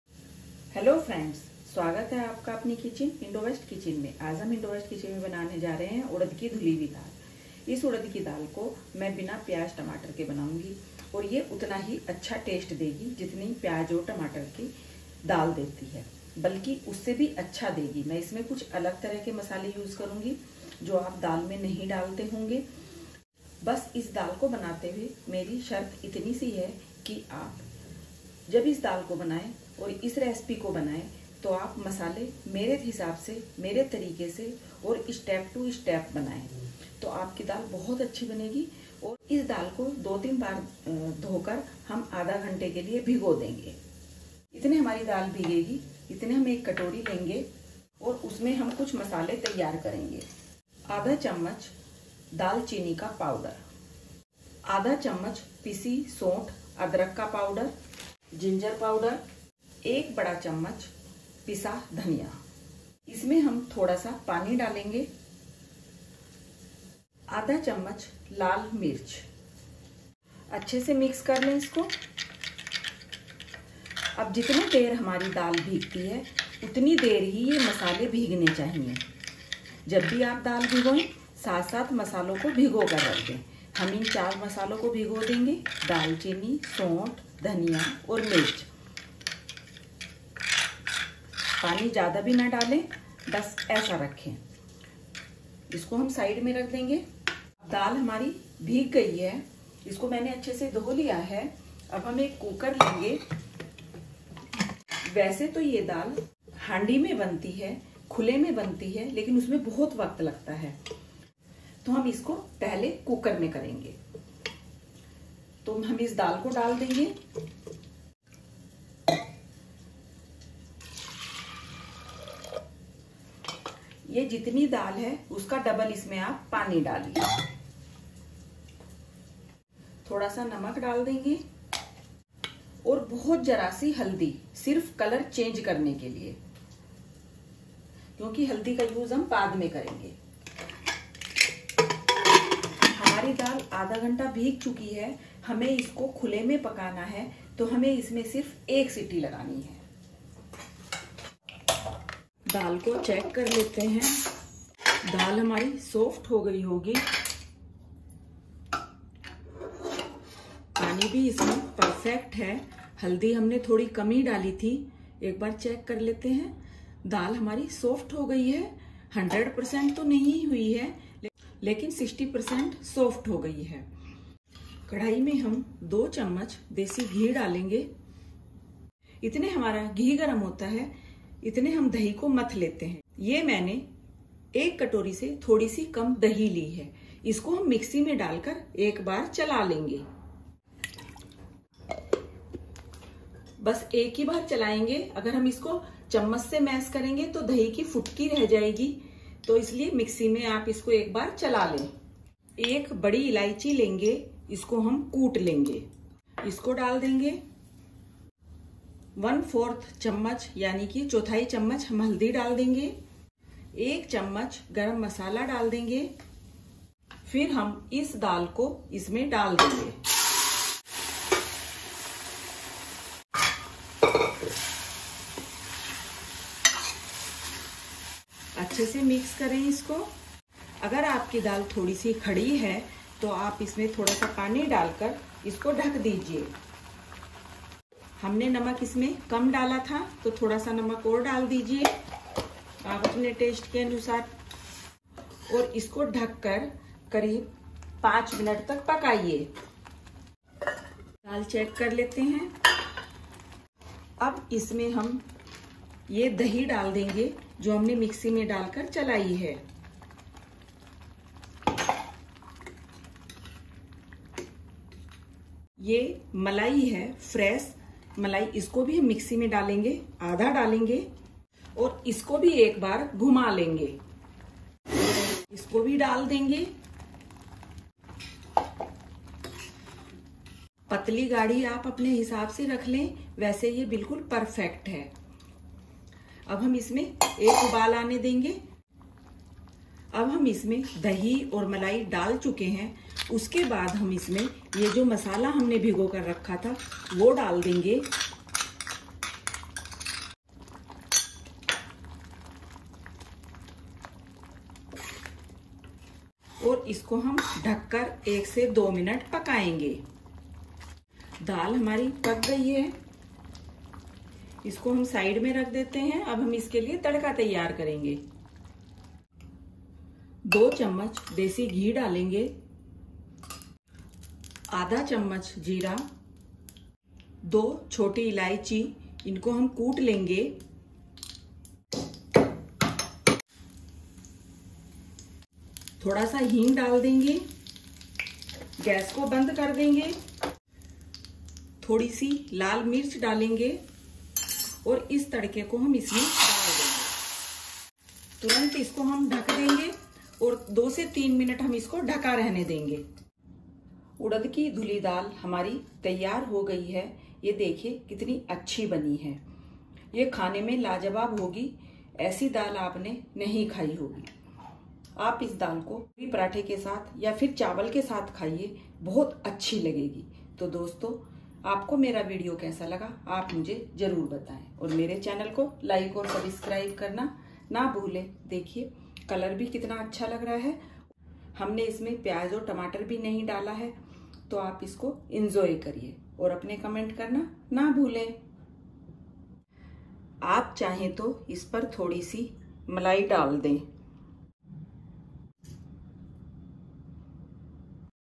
उड़द दाल,दाल तड़का खास ट्रिक और खास मसालों के साथ step by step method,90 साल पुरानी रेसिपी
कृपया वीडियो को अंत तक देखें || Please watch complete video